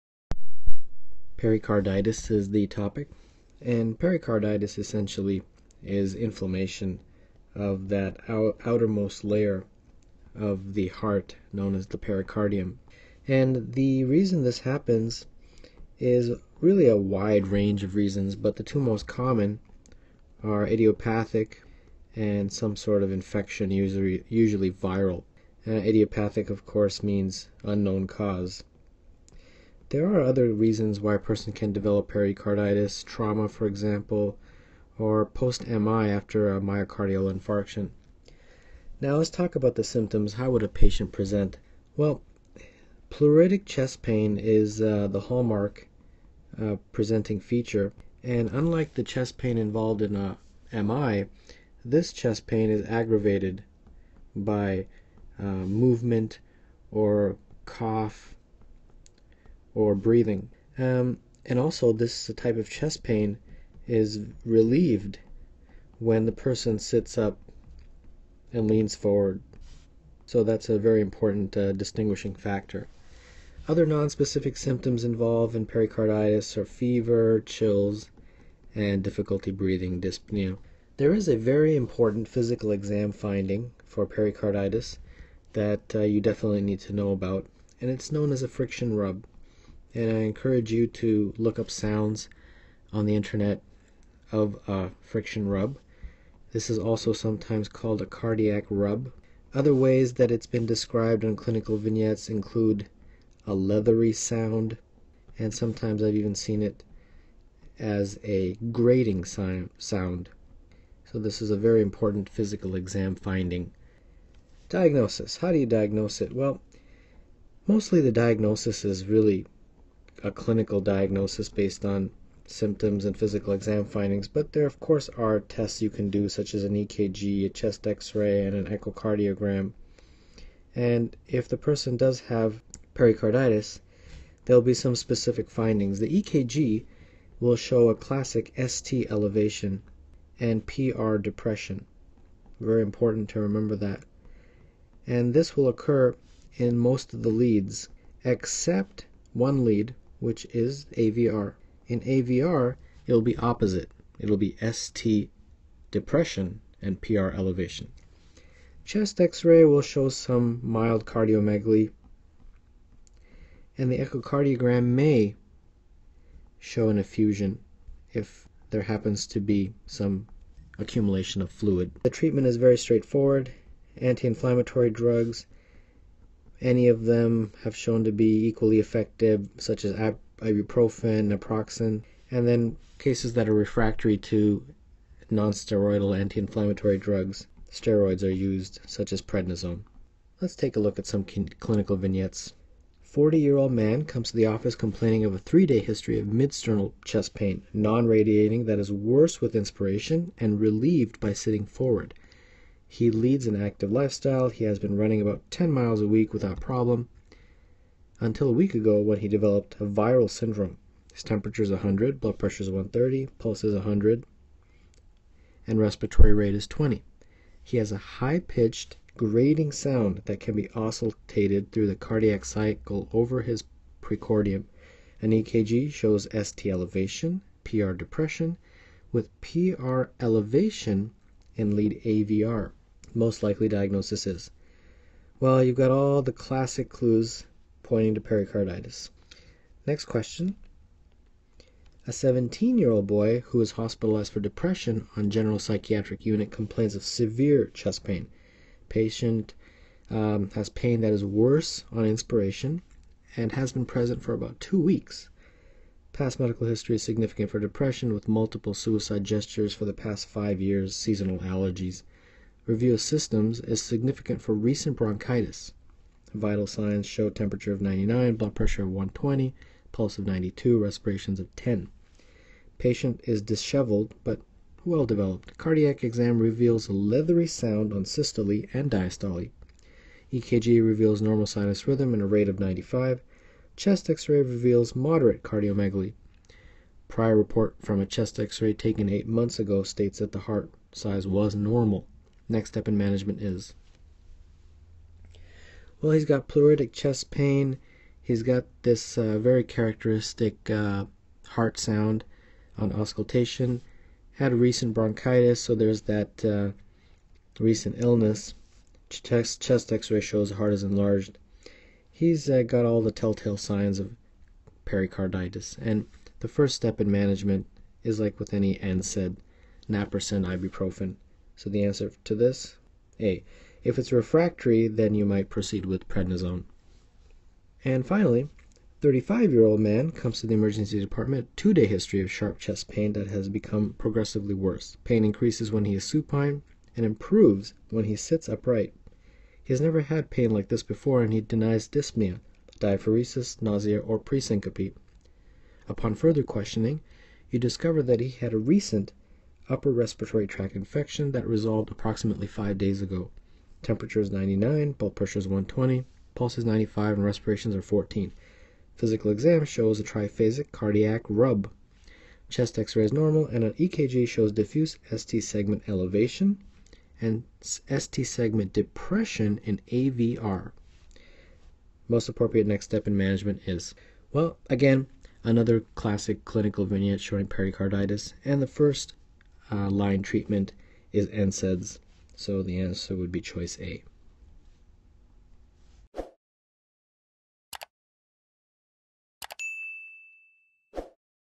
Pericarditis: Symptoms, ECG Findings & Treatment | High-Yield Clinical Cases | CanadaQBank
Pericarditis is a common cause of chest pain in clinical practice, but do you know how to recognize its classic features on ECG and manage it correctly? This high-yield medical video explains the symptoms, pathophysiology, and treatment of pericarditis, with real-world clinical vignettes to sharpen your exam prep.

What You’ll Learn in This Video:

What is Pericarditis? – Inflammation of the pericardium explained
Classic Symptoms – Sharp pleuritic chest pain, worse when lying flat, relieved by sitting forward
ECG Findings – Diffuse ST elevation, PR depression, and stages of ECG changes
Common Causes – Viral infection, autoimmune disease, post-MI (Dressler syndrome)
Diagnosis & Workup – ECG, echocardiography, inflammatory markers
Complications – Pericardial effusion, cardiac tamponade, constrictive pericarditis
Treatment Options – NSAIDs, colchicine, corticosteroids (in specific cases)
Clinical Vignettes – Test your ability to recognize and manage pericarditis in real-world scenarios

Help us continue to create free, high-yield content for learners around the globe.